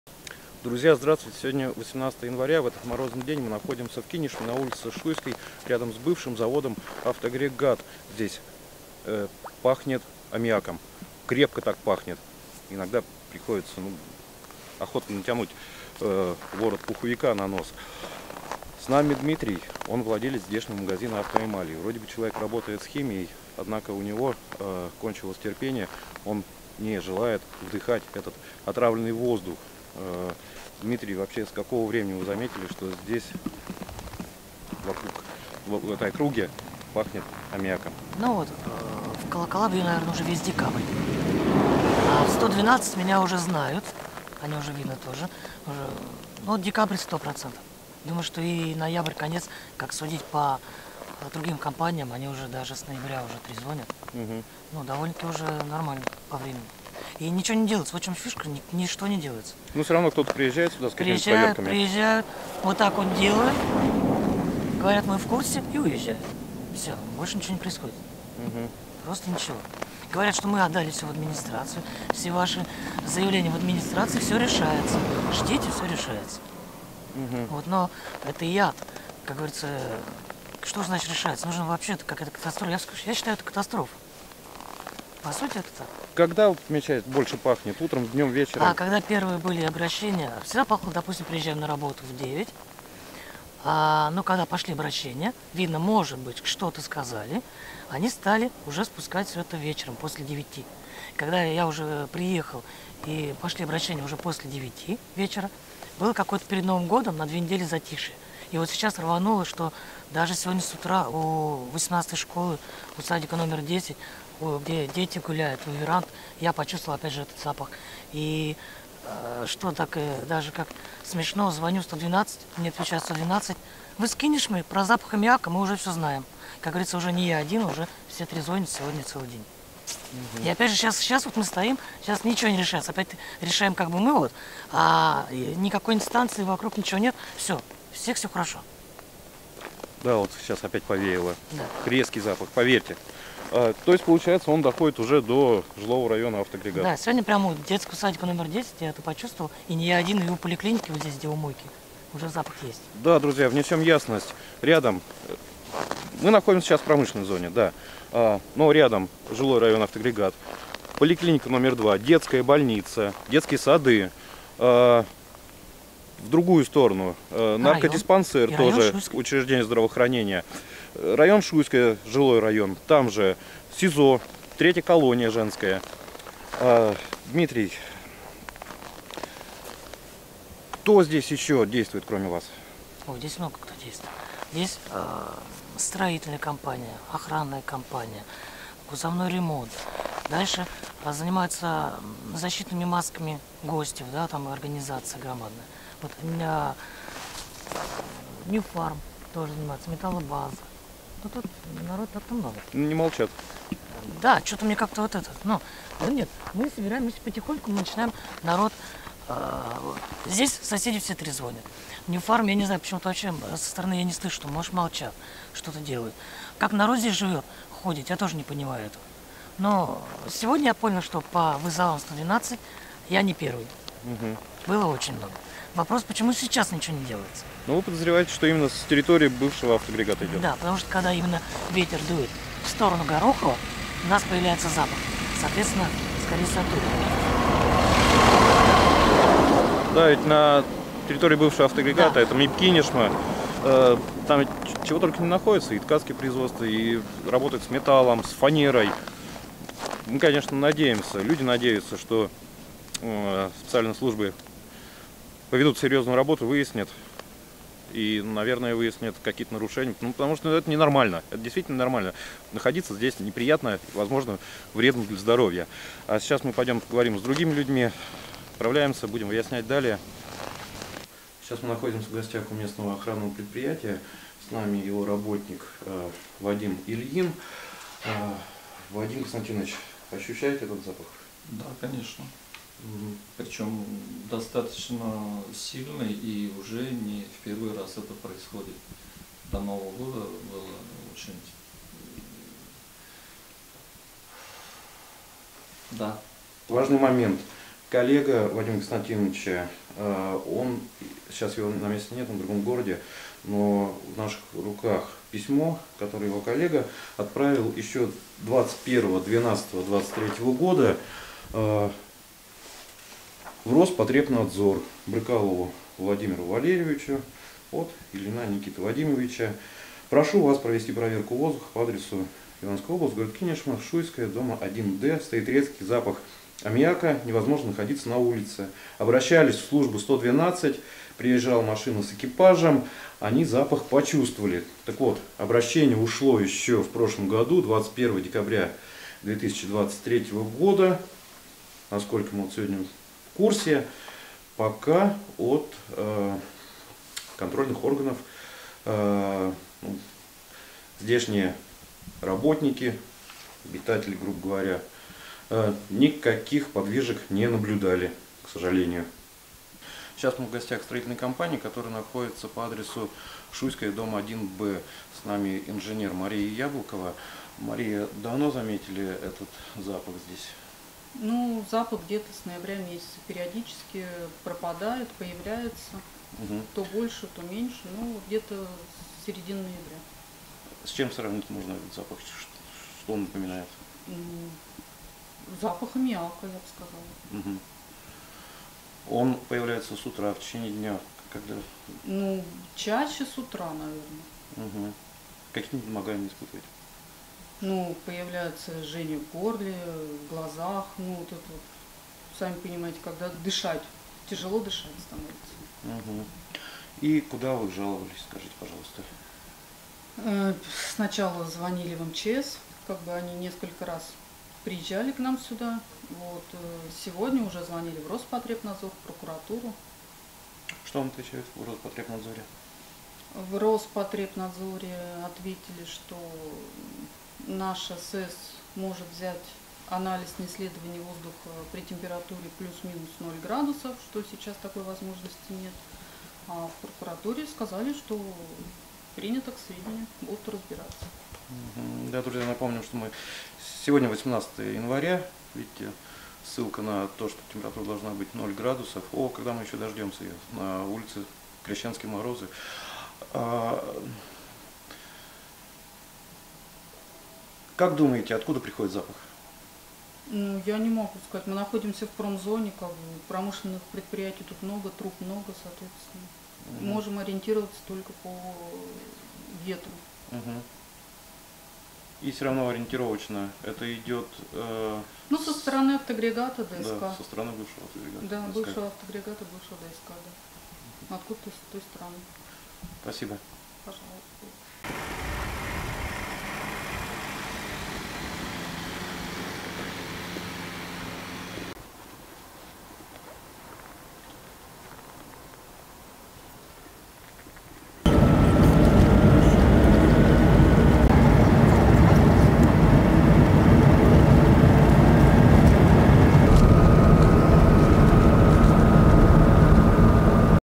В Кинешме над «Авдоагрегатом» стоит аммиачный духман
Люди жалуются, но им отвечают: «Чтобы исследовать воздух на аммиак, нужно потепление до 0 градусов». А на дворе пока 18 января. Крещенские морозы.
В редакцию «168 часов» обратились кинешемцы, которые не желают терпеть в воздухе отраву с аммиачным запахом. Ветер разносит её по окрестностям, предположительно, с территории бывшего завода «Автоагрегат», где сейчас действует индустриальный парк «Кинешма». Там работают с металлом, тканями, фанерой и с чем только не работают...